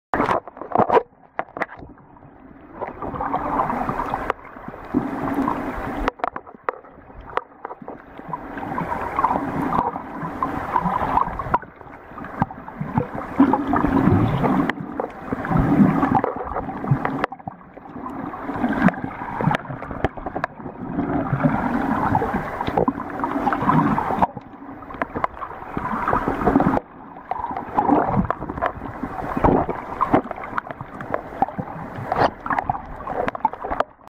Spotted Eagle Rays
Three Spotted Eagle Rays swimming passed me while snorkeling.